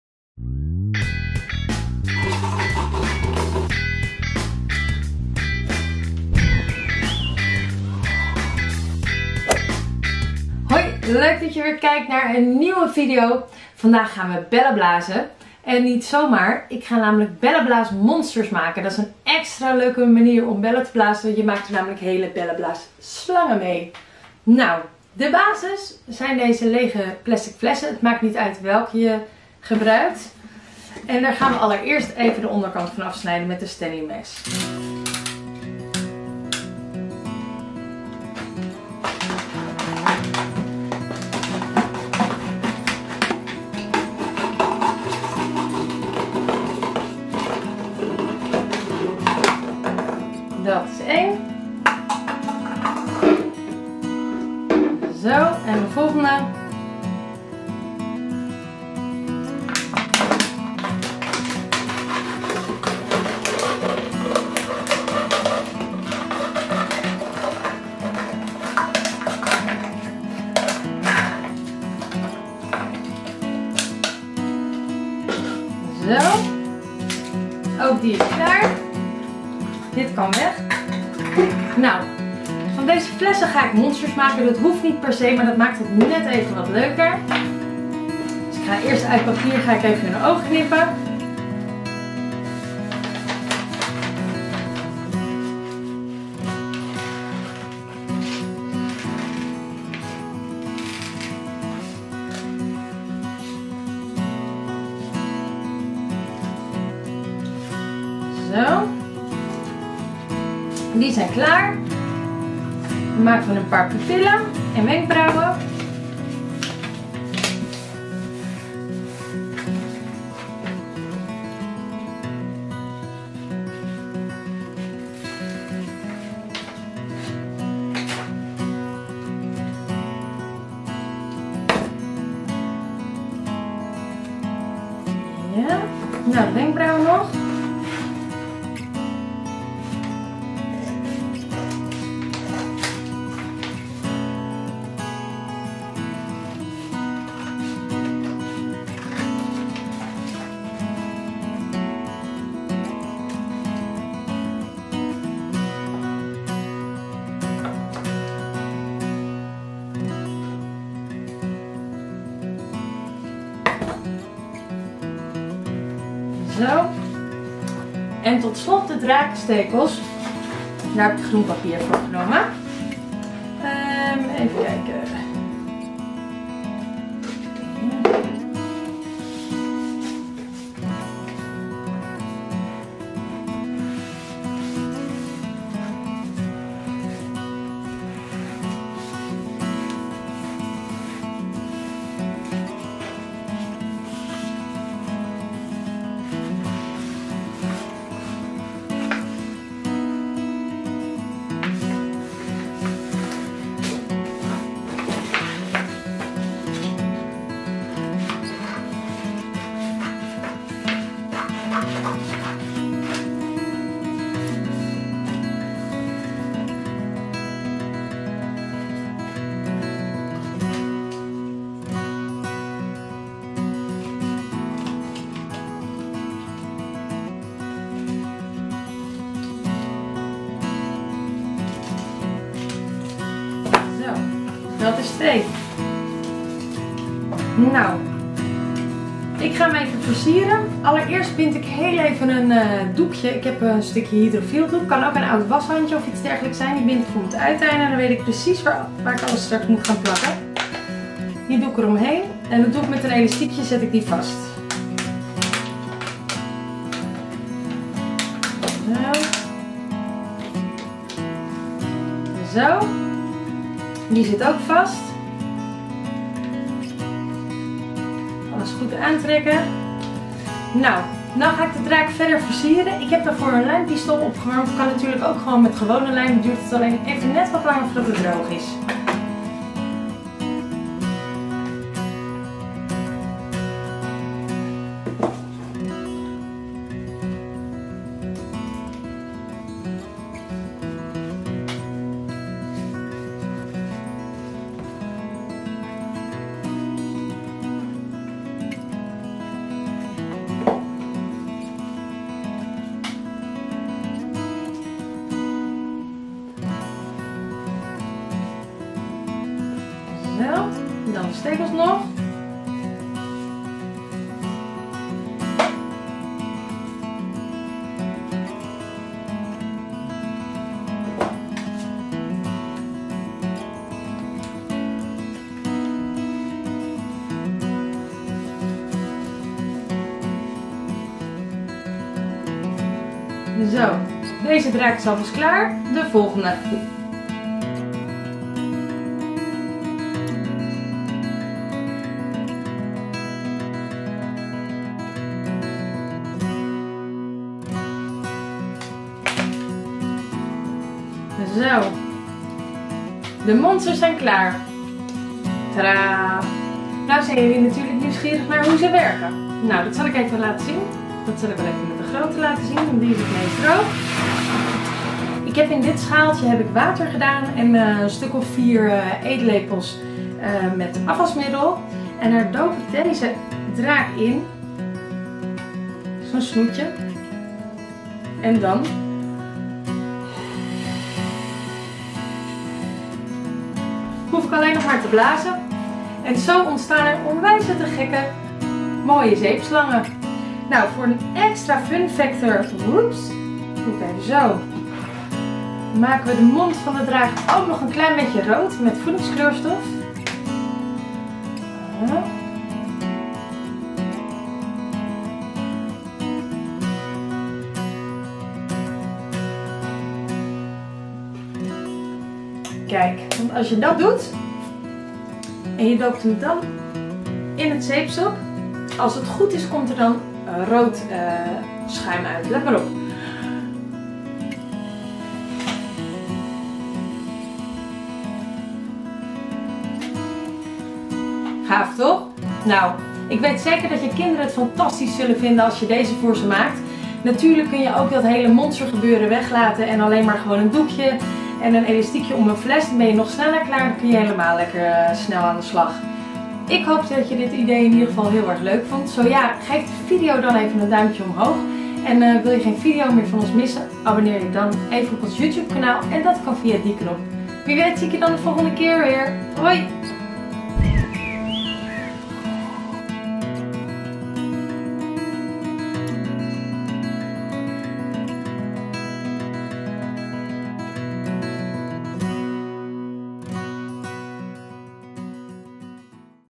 Bellen blazen met extra fun factor - Een zeepsop spugende draak maken
Met deze draak kunnen zelfs de kleinste kids nog bellen blazen. Je hebt er niet veel voor nodig: Alleen een lege fles, stukje stof (een oude washand of sok kan ook), een elastiekje en wat afwasmiddel. Check de video hoe je hem maakt en dan; blazen maar! Voor je het weet ligt de hele tuin vol met zeepsop slangen. Zo leuk! Kids love it!